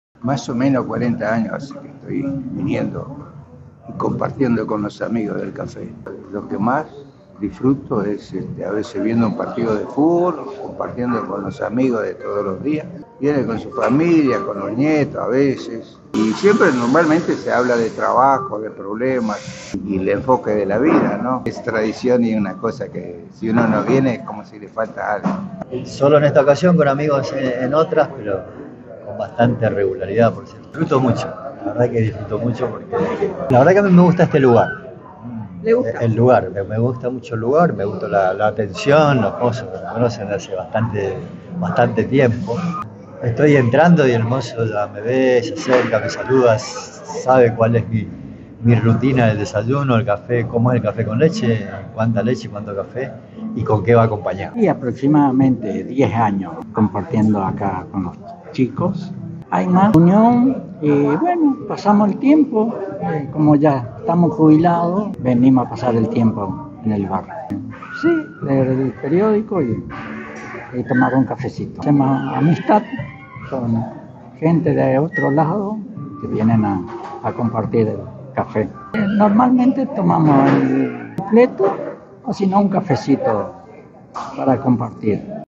Posadas | Compartir el café con amigos y comentar la actualidad sigue siendo una tradición
☕ En el centro de Posadas compartir el desayuno con amigos se ha convertido en una tradición profundamente arraigada. Cada mañana, grupos de amigos se reúnen para disfrutar de un buen café y conversar sobre la actualidad, fortaleciendo lazos sociales que han perdurado a lo largo de los años.